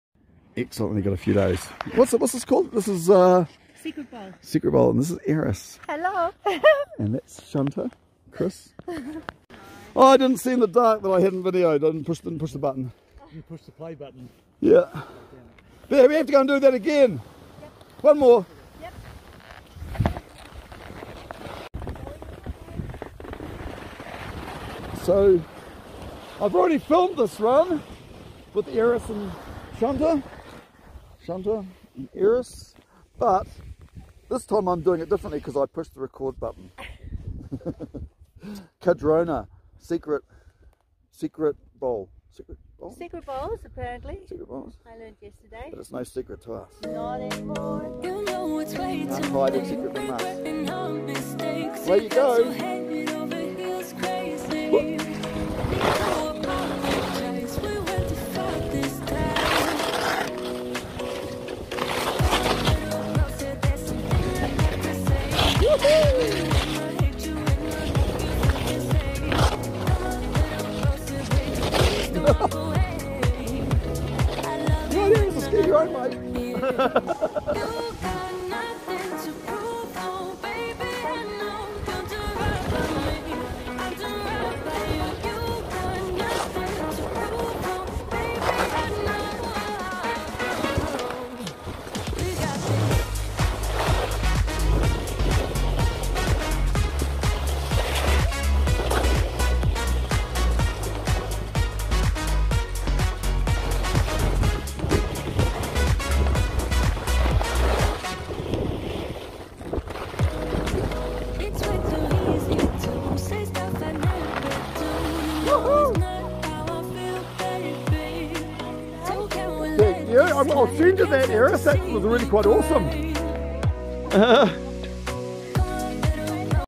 A day skiing at Cardrona ski resort for the first time in many years
I used to laugh about Cardrona Ski resort, saying it was great snow but the terrain is flat, I was young and wild then, now I'm older and the mountain seems to be steeper now is it me?